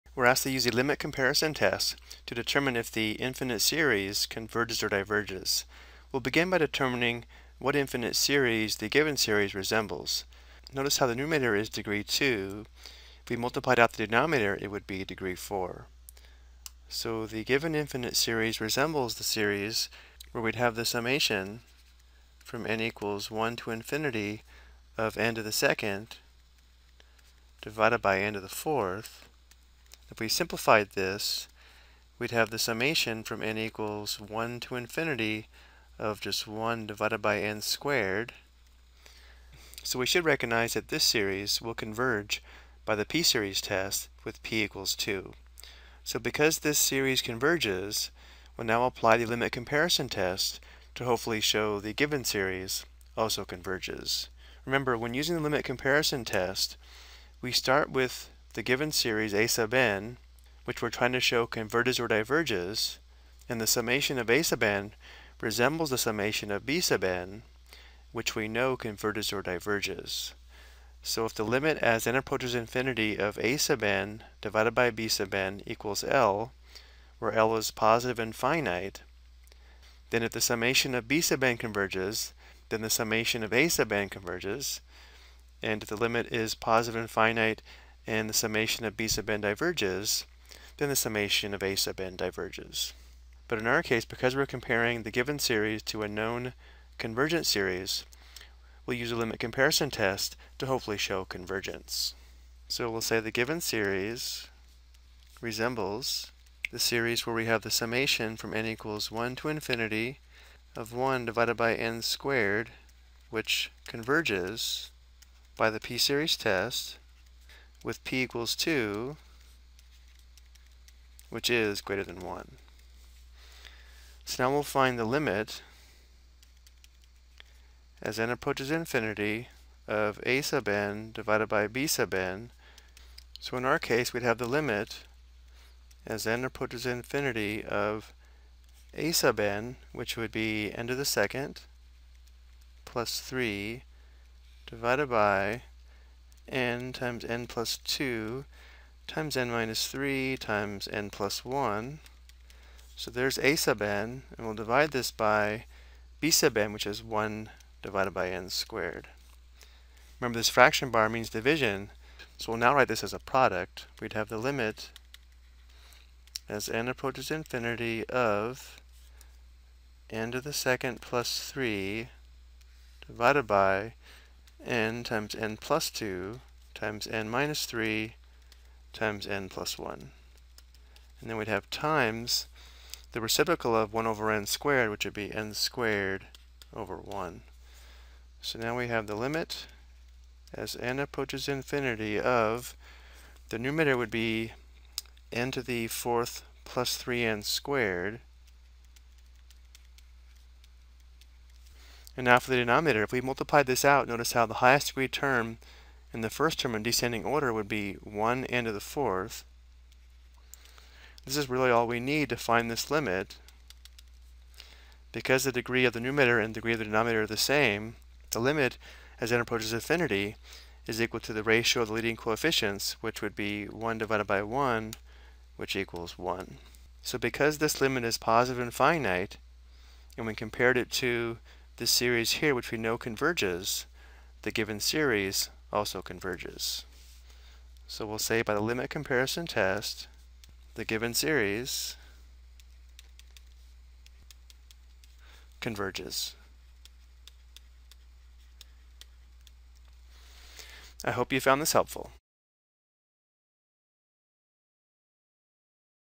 Ex: Infinite Series - Limit Comparison Test (Convergent)
This video provides an example of how to apply the limit comparison test to determine if an infinite series is convergent, divergent, or if the test is inconclusive.